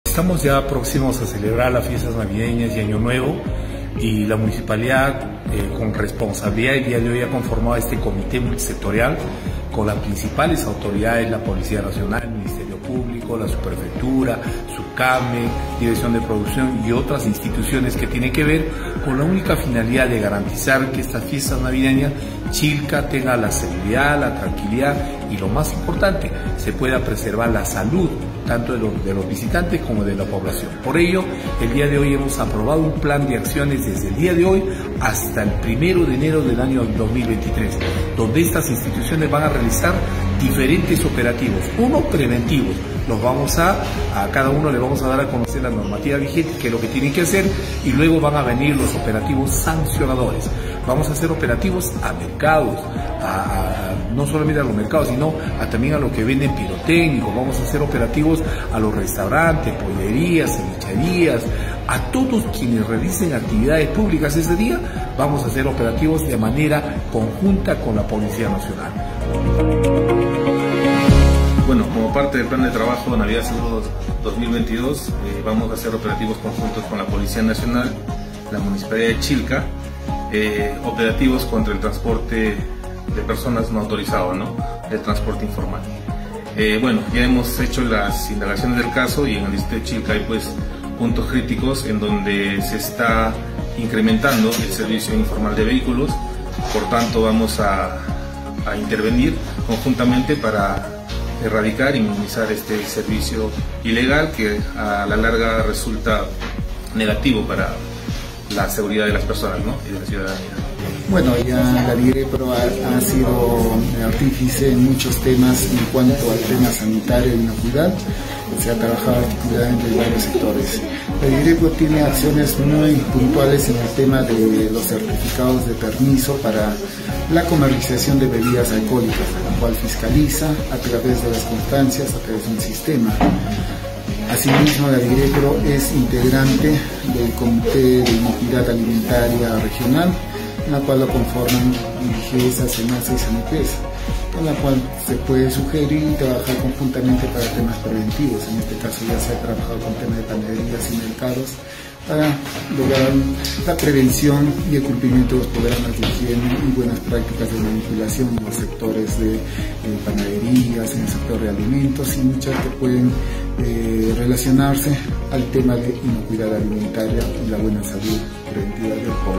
Comite multisectorial "Navidad segura Chilca"
Autoridades civiles y militares conforman comité multisectorial navidad segura Chilca 2022.